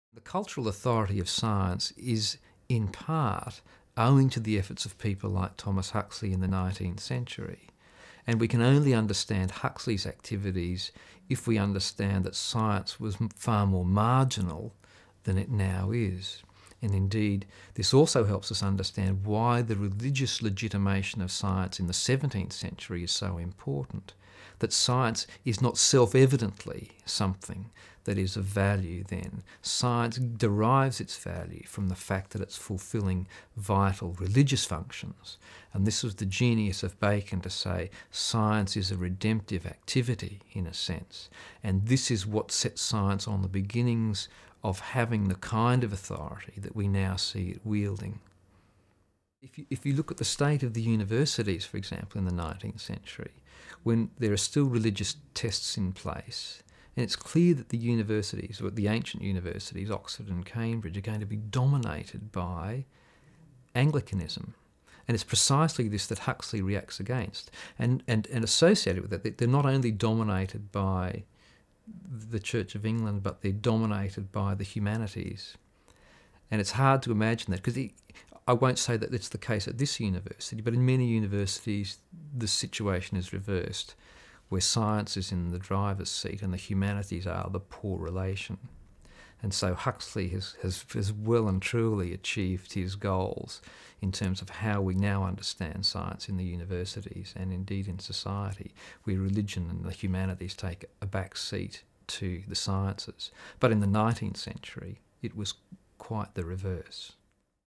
Why does Science have so much Cultural Authority?
Peter Harrison explains how the role of science in our culture has changed over time.